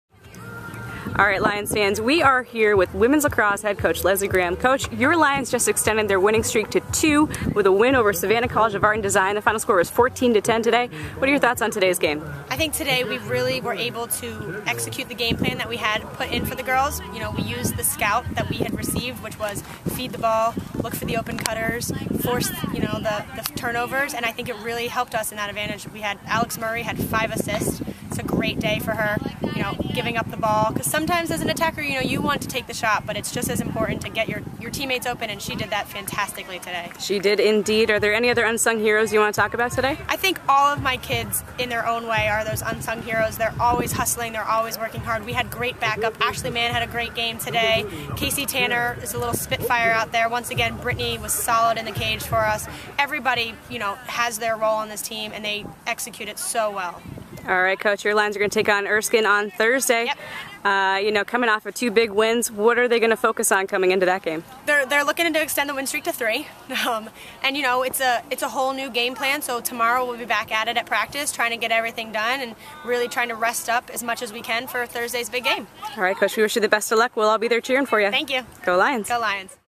Women's Lacrosse Postgame 03-20-12
Head Coach Lesley Graham talks about the women's lacrosse team's second-straight victory after the Lions' defeated SCAD on Tuesday evening at University Field.